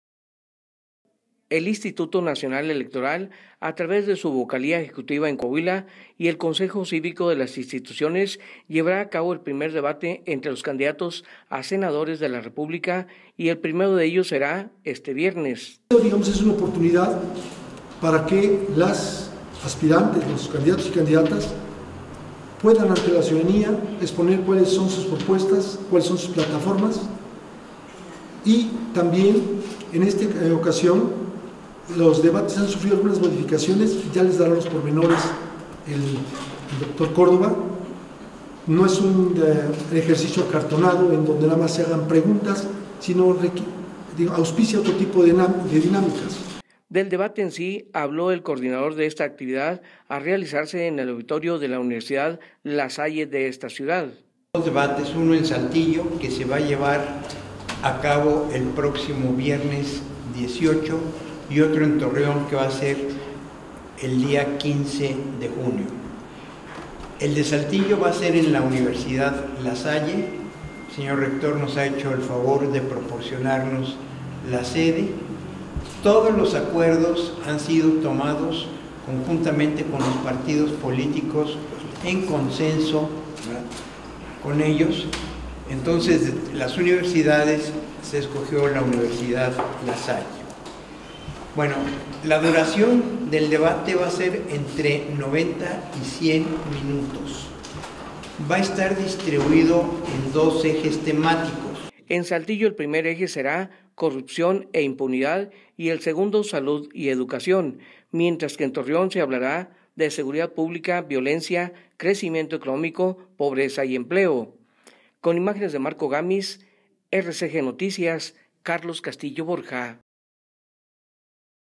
Primer debate de candidatos a Senadores
El Instituto Nacional electoral a través de su vocalía ejecutiva en Coahuila y el Consejo Cívico de las Instituciones, llevará a cabo  el primer debate entre los candidatos a Senadores de la República y el primero de ellos será este viernes.